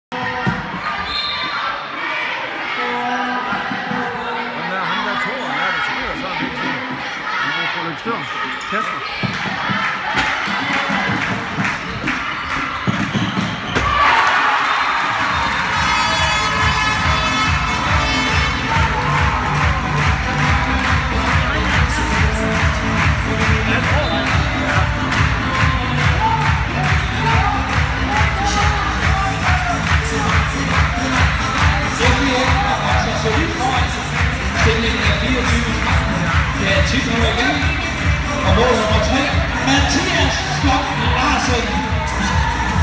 Oprykningskamp
1. hold herre spiller mod Farsø KFUM HK om den direkte oprykning til Jyllandsserien